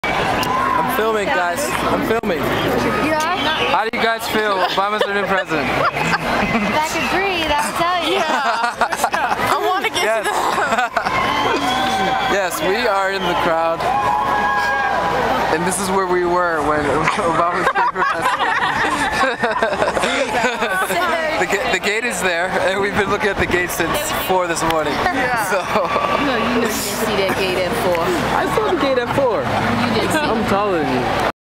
Where I was when Obama became President
I went down to DC with a few friends to witness the swearing in of our 44th president.  We were told that the entrance at 7th & D was to get to the mall.  After standing in line for a while it became apparent that we were going to miss the inauguration.  It turns out the "line" we were in was for the inaugural parade!  Anyway, I filmed this just before then president-elect Obama was sworn in.